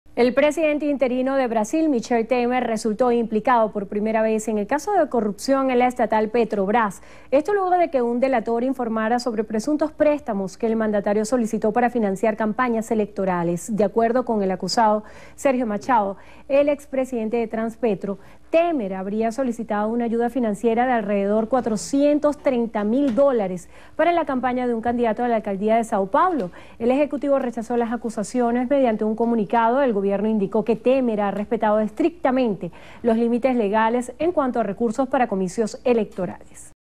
Presidente interino de Brasil implicado en caso Petrobras
El presidente interino de Brasil, Michel Temer resultó implicado por primera vez en el caso de corrupción en la estatal Petrobras. Esto, luego de que un delator informara sobre presuntos préstamos que el mandatario solicitó para financiar campañas electorales.